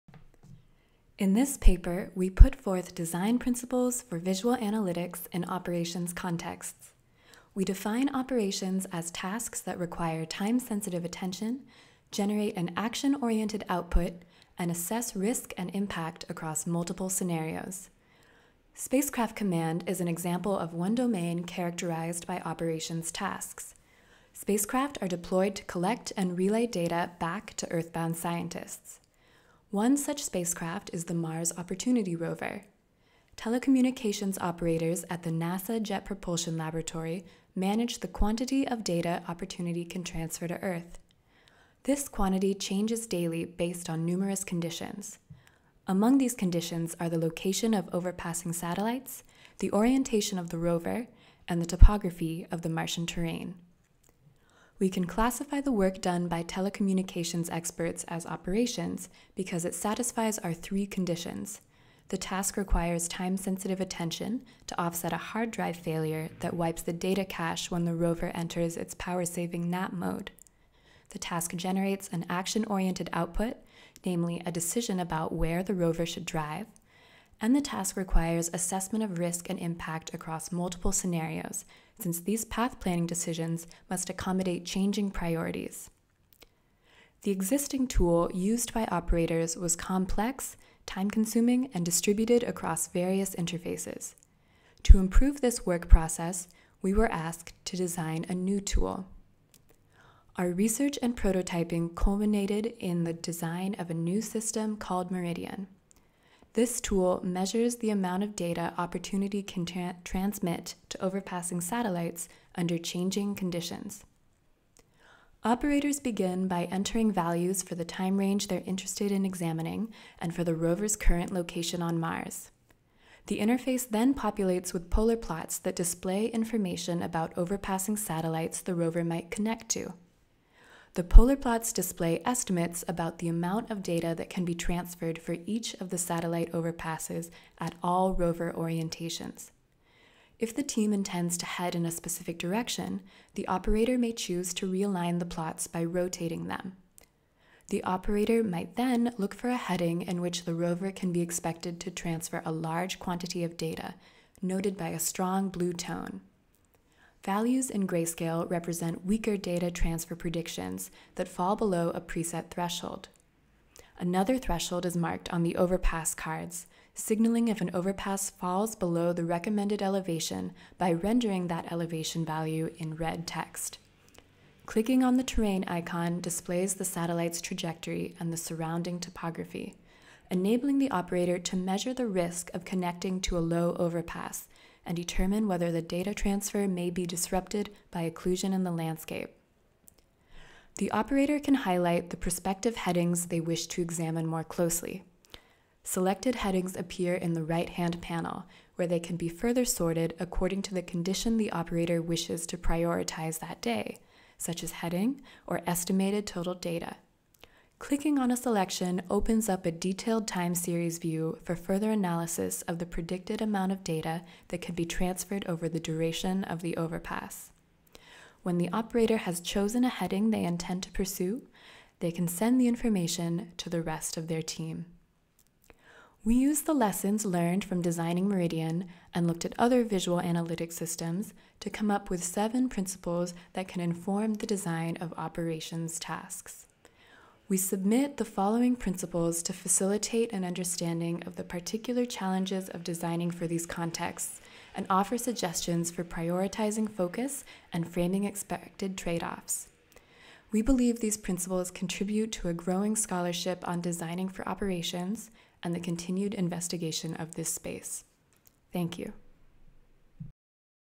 Towards Design Principles for Visual Analytics in Operations Contexts
Operations engineering teams interact with complex data systems to make technical decisions that ensure the operational efficacy of their missions. To support these decision-making tasks, which may require elastic prioritization of goals dependent on changing conditions, custom analytics tools are often developed. We were asked to develop such a tool by a team at the NASA Jet Propulsion Laboratory, where rover telecom operators make decisions based on models predicting how much data rovers can transfer from the surface of Mars. Through research, design, implementation, and informal evaluation of our new tool, we developed principles to inform the design of visual analytics systems in operations contexts. We offer these principles as a step towards understanding the complex task of designing these systems. The principles we present are applicable to designers and developers tasked with building analytics systems in domains that face complex operations challenges such as scheduling, routing, and logistics.

Presented at the 2018 CHI Conference on Human Factors in Computing Systems (ACM CHI 2018).

Matthew Conlen, Sara Stalla, Chelly Jin, Maggie Hendrie, Hillary Mushkin, Santiago Lombeyda, Scott Davidoff